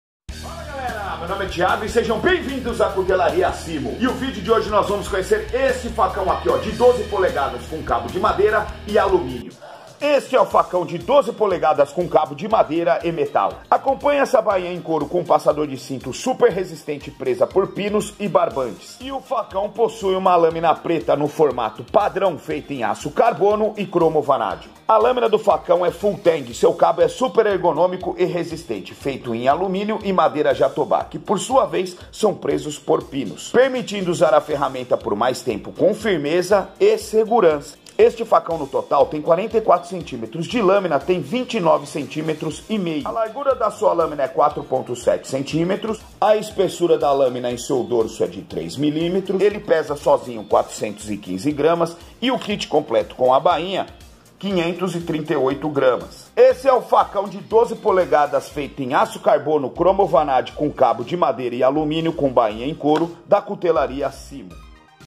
FACÃO 12 POLEGADAS COM CABO DE MADEIRA E ALUMÍNIO [MINUTO CIMO]
FACÃO CIMO 12” CARBONO CABO ALUMÍNIO E MADEIRA COM BAINHA - 1070/12 C/B

Facão lâmina aço Carbono Cromo-vanádio com pintura especial para atingir alto grau de cobertura e proteção corrosiva. Cabo em Alumínio injetado em alta pressão / inserto de Madeira Muracatiara.

Acompanha bainha de couro. Perfil “full tang” ou seja lâmina integral: lâmina e cabo em uma peça só. Lâmina com afiação final, pronta para uso.

DESCRIÇÃO TÉCNICA
Comprimento da Lâmina (cm)
30.48

Espessura da Lâmina (mm)
1.75

Cor da Lâmina
Preto

Tratamento Térmico
Temperado e revenido

Tipo de Afiação
Hollow Grind

Material da Lâmina
Aço Cormo Molibdênio Vanádio

Dureza da Lâmina (HRc)
46 – 48

Acabamento da Lâmina
Pintado

Tipo de Lâmina
Facão

Comprimento do Cabo (cm)
14.2

Material do Cabo
Alumínio / Madeira

Peso do Produto (gr)
492

Sistema de Fixação do Cabo
Rebites

Bainha
Com Bainha